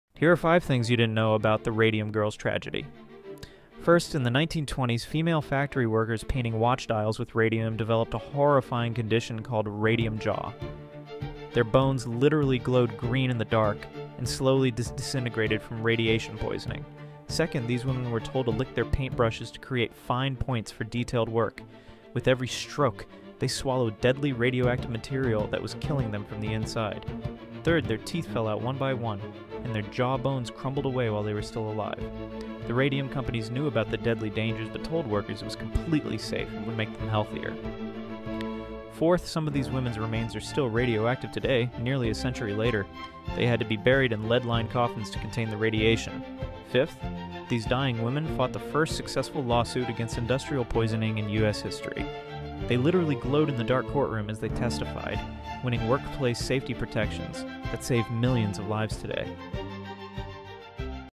Factory Workers Literally Glowed Green Before Dying
The Radium Girls tragedy changed everything